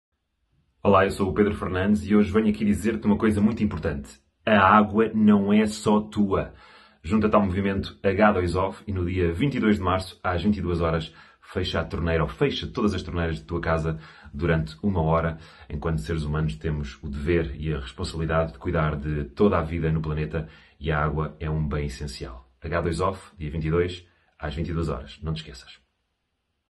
Embaixadores da água - Pedro Fernandes APDA​ H2Off​ CECEA​
A Hora da água está a chegar e conta já com dezenas de embaixadores ligados ao mundo da música, do cinema, da cultura, da gastronomia ou do futebol.

Junte-se a nós, dia 22 de março, às 22 Horas … porque juntos fazemos a diferença!